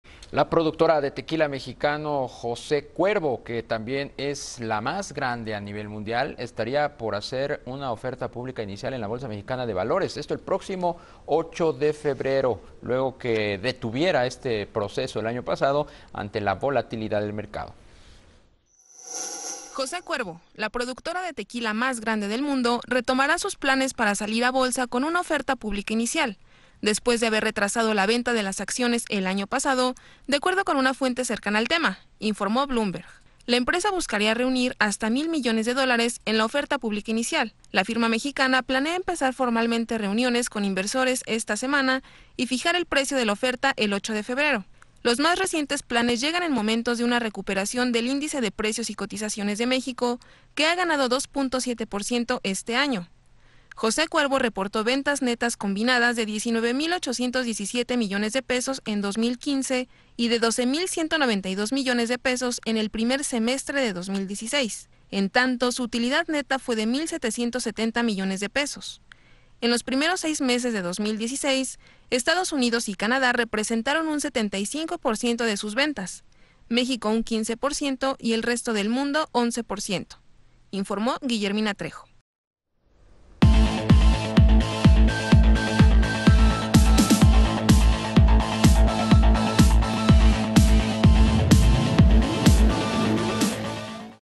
José Cuervo podría cotizar en la BMV en las próximas semanas
La productora de Tequila mexicano José Ciervo, que también es la más grande a nivel mundial, estaría por hacer una oferta pública inicial en la Bolsa Mexicana de Valores. Esto el próximo 8 de febrero, luego de detener este proceso el año pasado ante la volatilidad del mercado.

24 enero de 2017